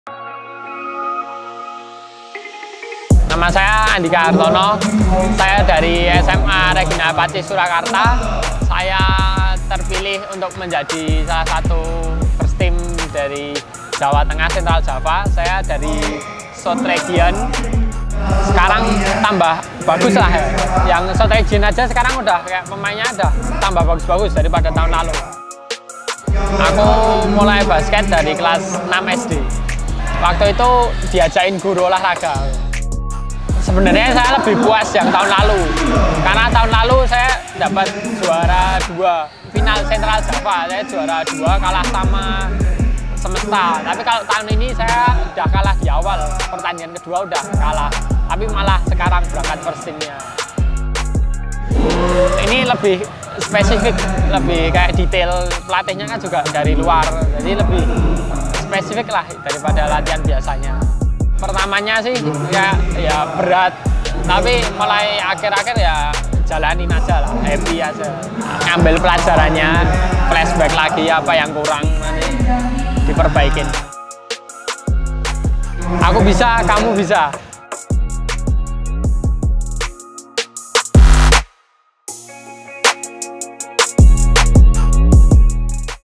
Tahun Kedua Yang Istimewa Bagi Andika Hartono
Walaupun tim basket sekolahnya sudah tersingkir terlebih dahulu di babak awal, Andika Hartono berkesempatan untuk menjadi First Team Honda DBL Central Java 2018 dan berangkat ke Surabaya untuk menjadi peserta Honda DBL Camp 2018. Bagaimanakah kisah Andika untuk memperoleh kesempatan menjadi peserta Honda DBL Camp di tahun keduanya?

Selengkapnya hanya di Mainbasketplus.